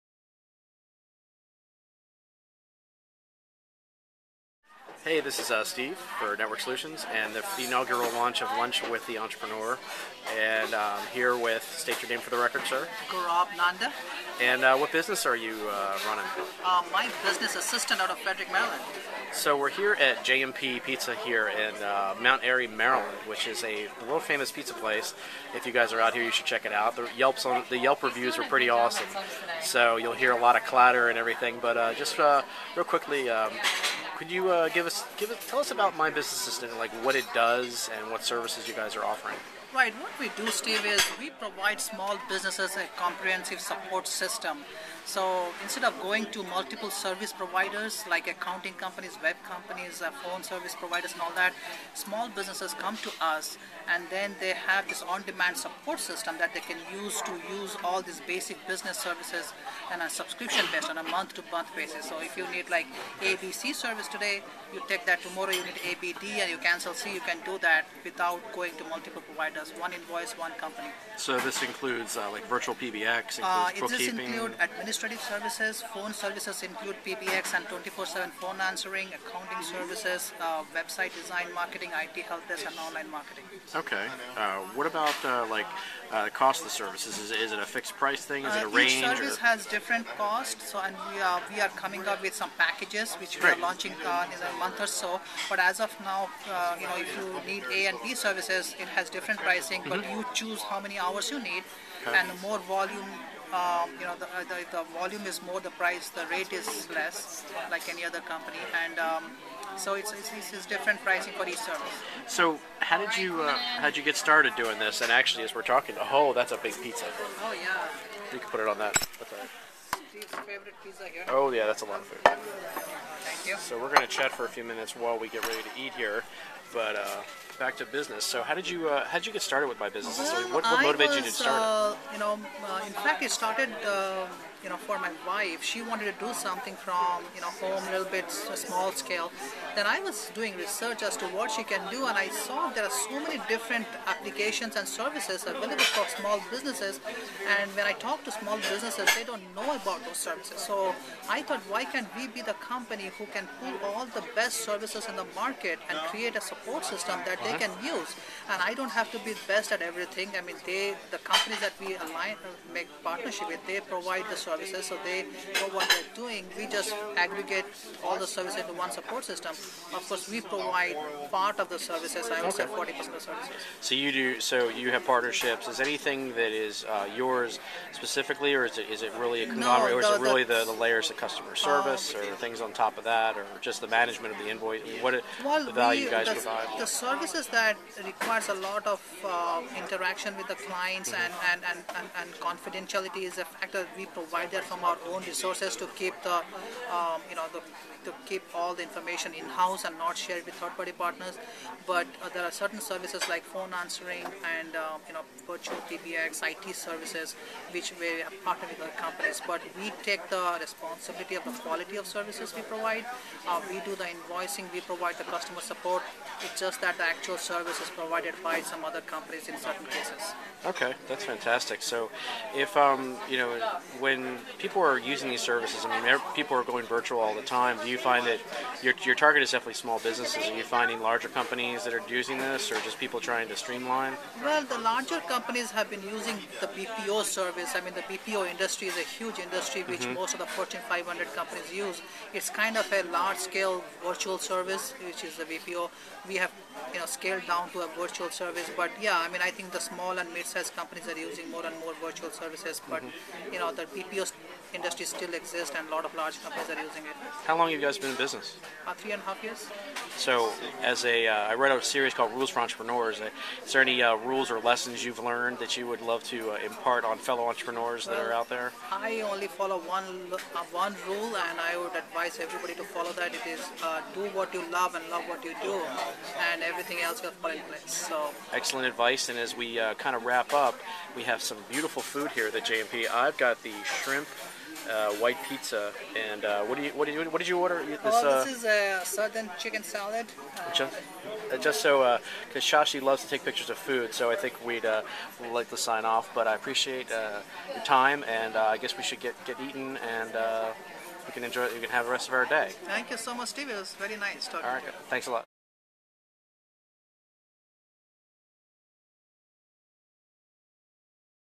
"Lunch with Entrepreneurs" #1 - Gourab Nanda
Interview with Gourab Nanda of MYBusinessAssistant.com at J&P Pizza in Mt. Airy, MD.